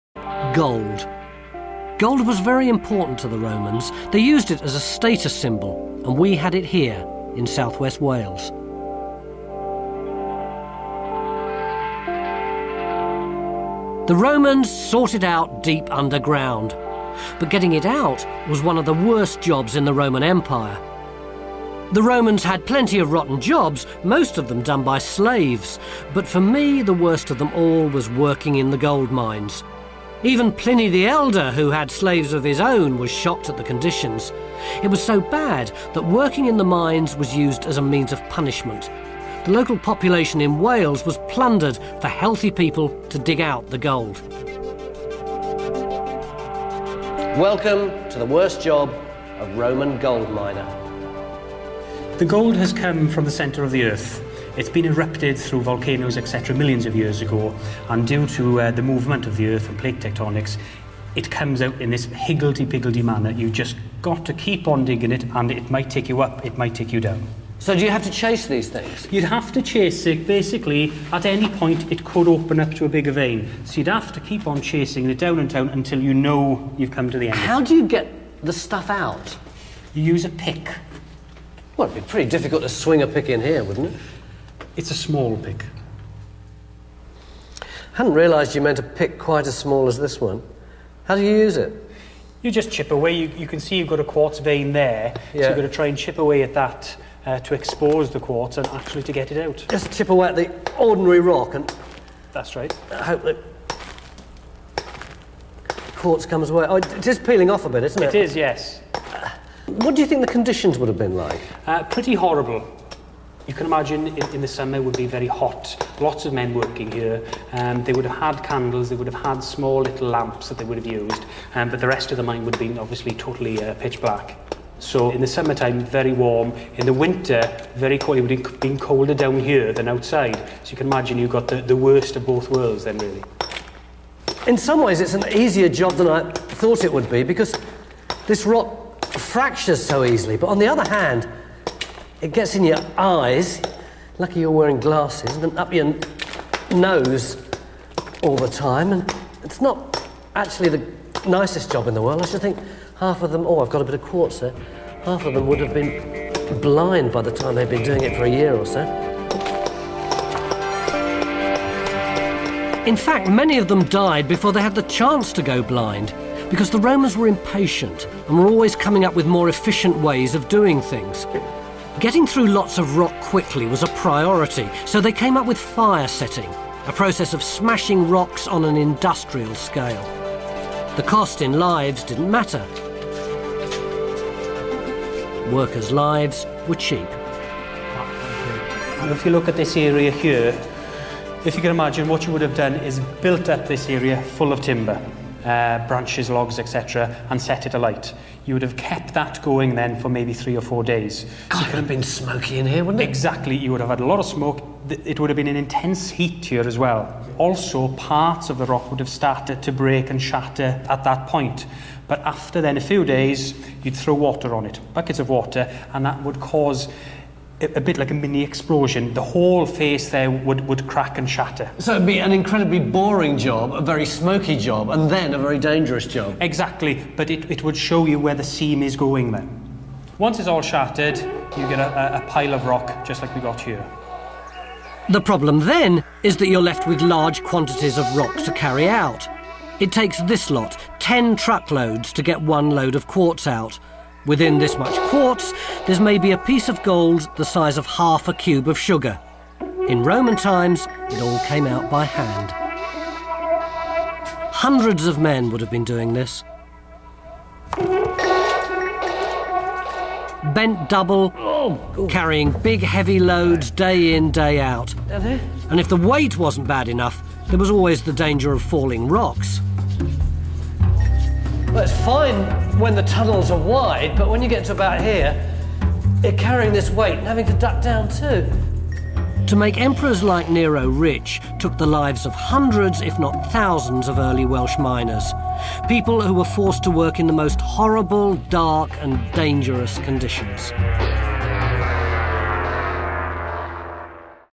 Making History - Medieval Mining
Since the Bronze age the strength and security of a civilization depended on their access to various metals such as gold, silver, copper, tin, and Iron.  Finding and extracting these metals proved to be extremely difficult